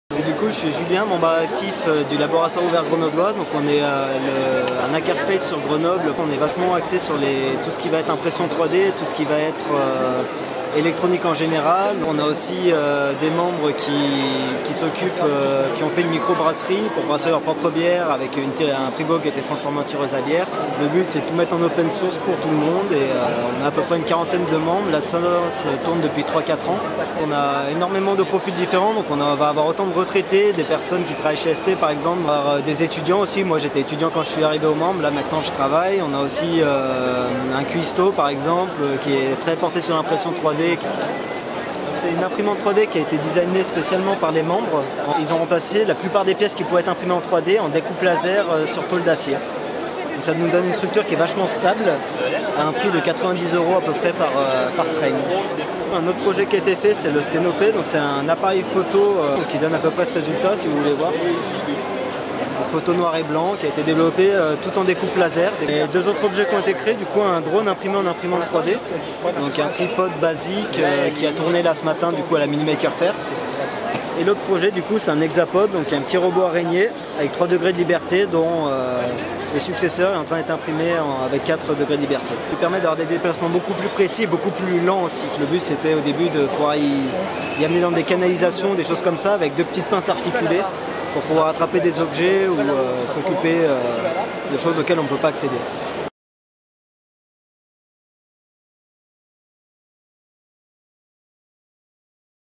Le Laboratoire Ouvert Grenoblois, le hackerspace de Grenoble
Julien, du LOG (laboratoire ouvert grenoblois), présente le hackerspace de Grenoble et les projets de ses membres, particulièrement portés sur l'impression 3D et l'électronique, à l'occasion de la 1ère Mini Maker Faire Grenoble.